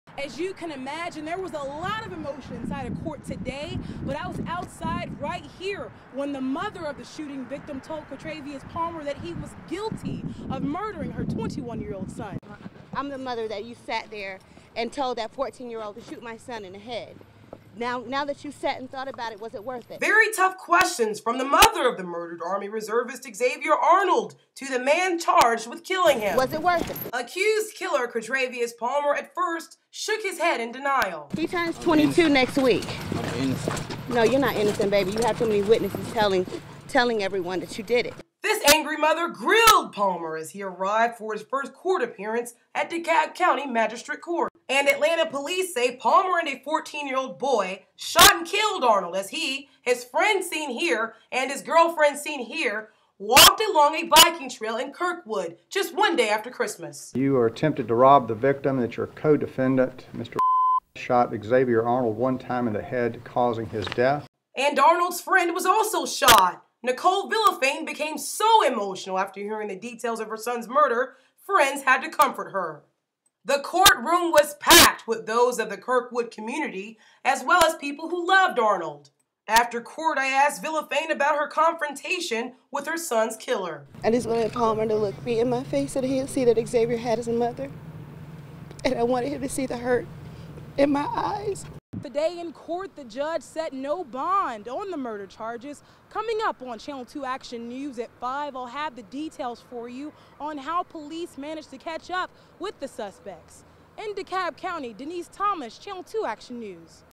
Denise Thomas Reporter Reel 2014 Package
21 Year old Xavier Arnold was shot and killed and here in the video shows his mother Nicole Villafane, confronting one of the accused killers, 22 year-old Quatravius Palmer. Allegedly, Palmer stood by